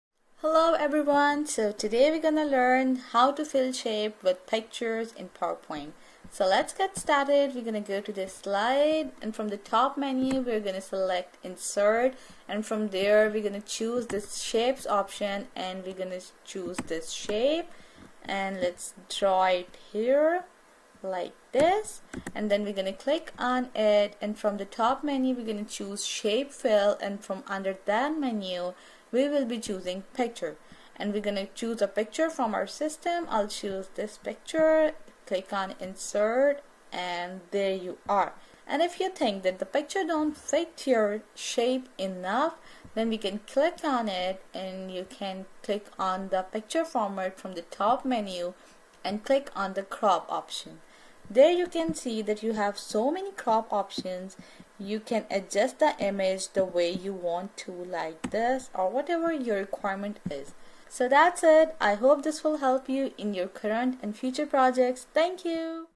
How to Fill Shape with a Picture in PowerPoint
You can fill objects and shapes with pictures in Microsoft PowerPoint. First, draw your shape, it can be any shape! Circle, oval, square, rectangle, triangle, pentagon, polygon or anything in between. Then, apply your image as the texture on the object. You can adjust the image inside the object by using the crop tool.